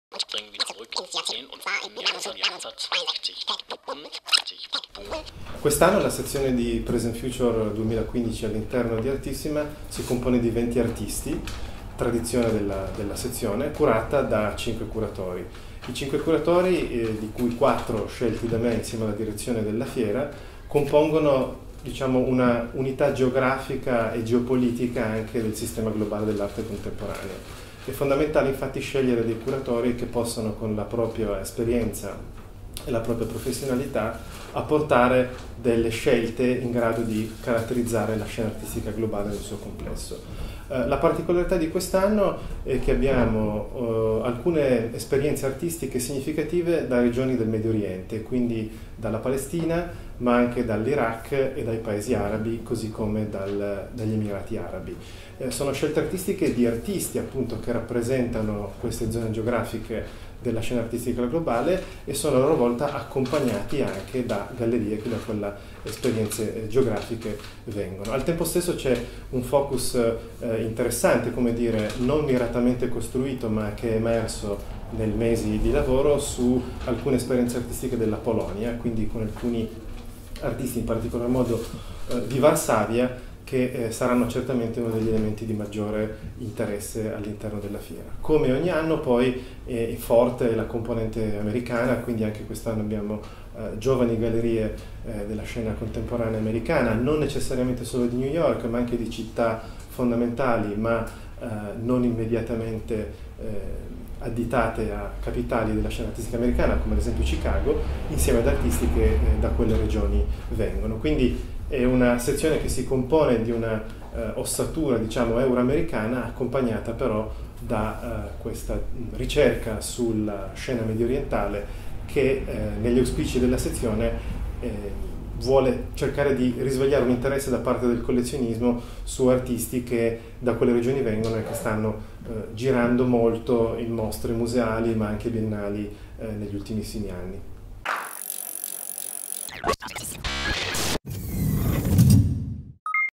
Artissima 2015 // Present Future - Intervista a Luigi Fassi
ARTISSIMA 2015 - Preview
Intervista a Luigi Fassi, coordinatore della sezione Present Future.